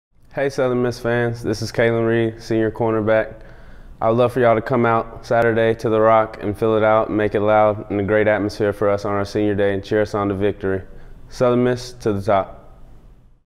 SelloutSaturday message from Kalan Reed
SelloutSaturday at The Rock this Saturday at 2:30 p.m. for the last regular season game of the year! We need a huge crowd to root on our Golden Eagles to victory! Your Golden Eagles are 7-3 and tied for 1st in the Western Division standing in C-USA.